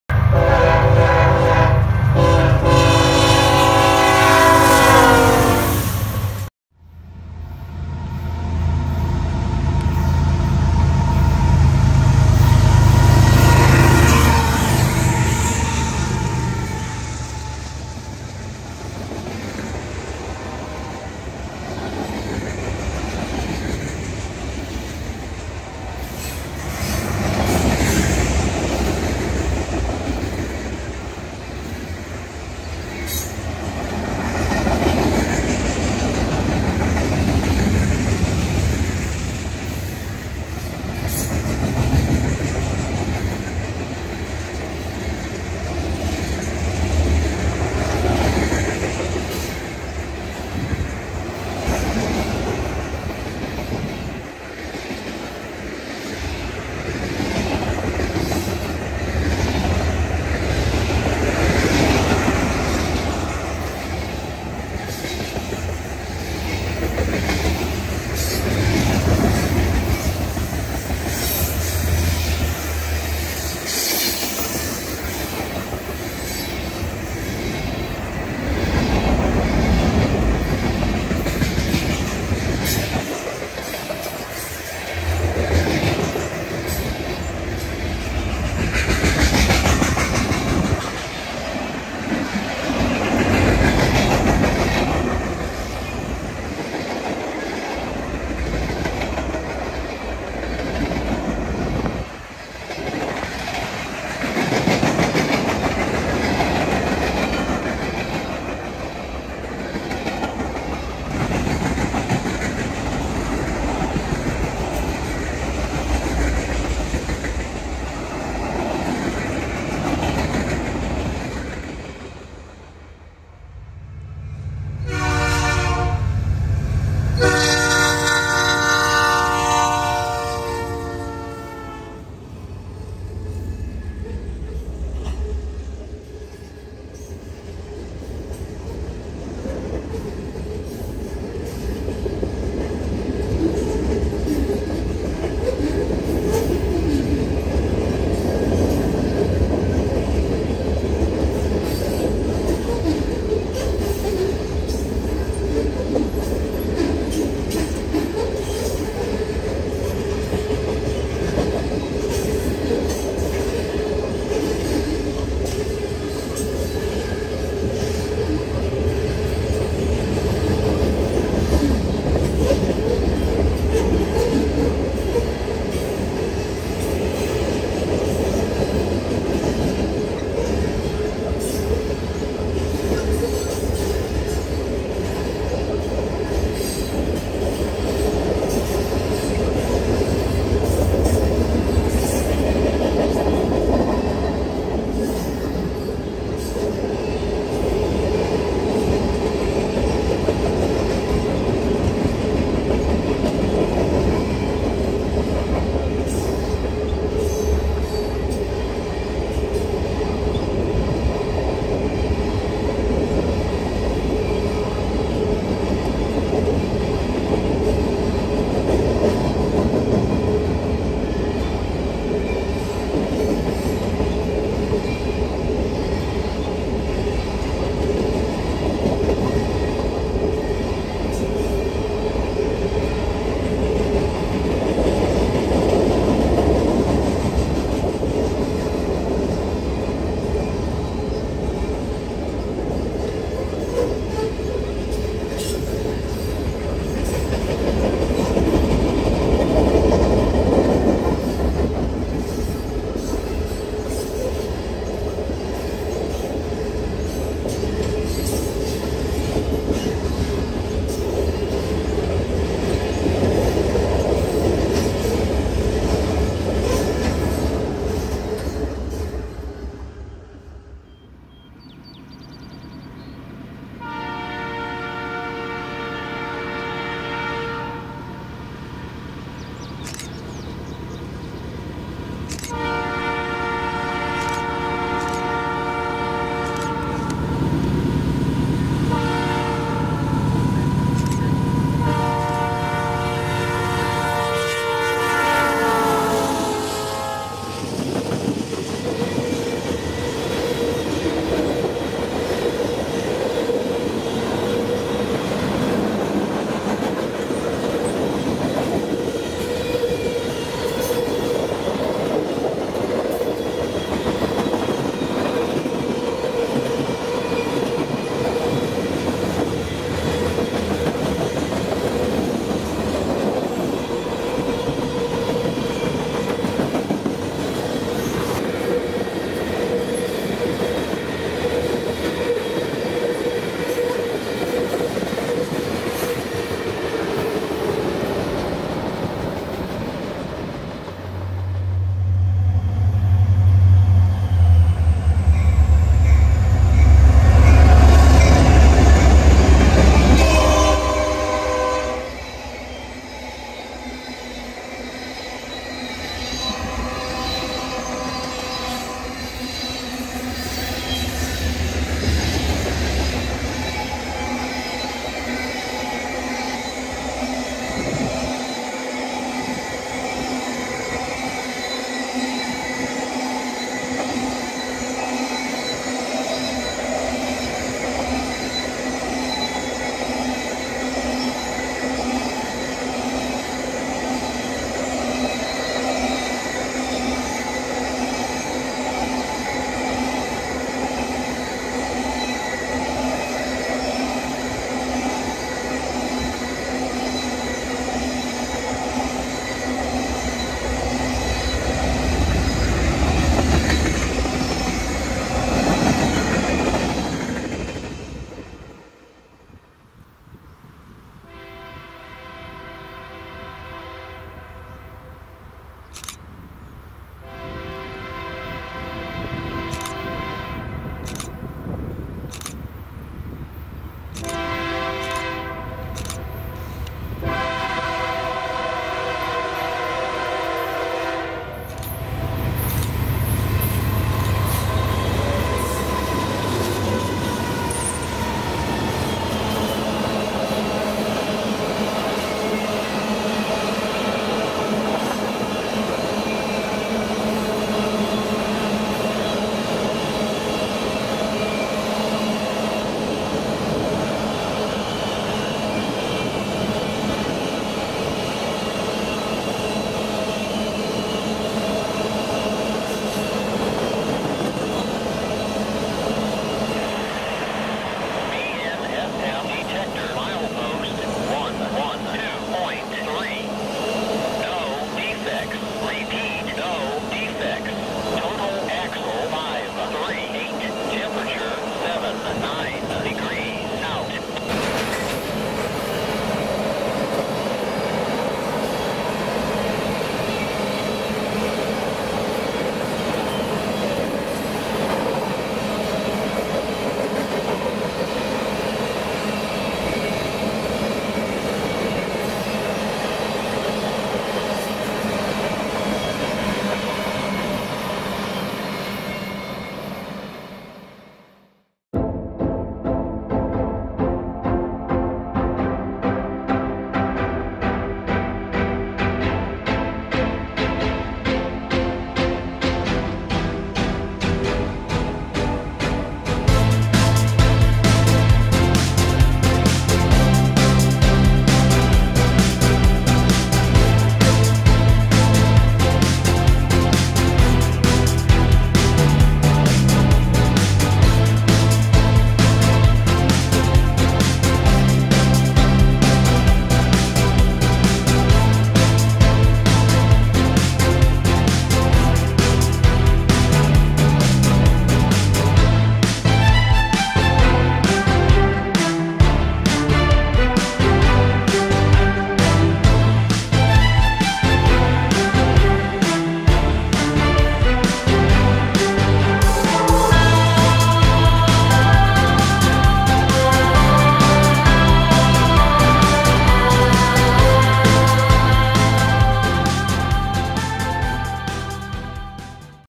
Nice Mix of Different Railroads on the Hinckley Sub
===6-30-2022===
Train: MITDM (Manifest) 9:46 AM
Unit 1: UP 7991
Unit 2: UP 7357

===7-3-2022===
Train: H-SUPNTW (Manifest) 5:49 PM
Unit 1: BNSF 251
Unit 2: BNSF 252
Unit 3: BNSF 4157

===7-4-2022===
Train: CP 251 (Manifest) 8:46 PM
Unit: CP 8529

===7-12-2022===
Train: Hinckley Local (Manifest) 2:08 PM
Unit 1: BNSF 1833
Unit 2: BNSF 1943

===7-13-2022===
Train: U-MADALL (Taconite Empty) 4:16 PM
Unit 1: BNSF 5207
Unit 2: BNSF 3952
Unit 3: BNSF 4125